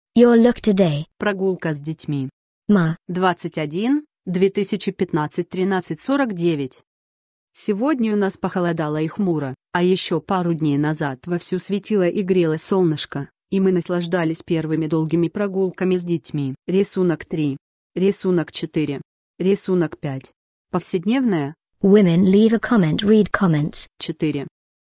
LJ150321 351 your look today прогулка с детьми
your look today прогулка с детьми Mar 21, 2015 13:49 сегодня у нас похолодало и хмуро, а еще пару дней назад вовсю светило и грело солнышко, и мы наслаждались первыми долгими прогулками с детьми. повседневное, women Leave a comment Read comments 4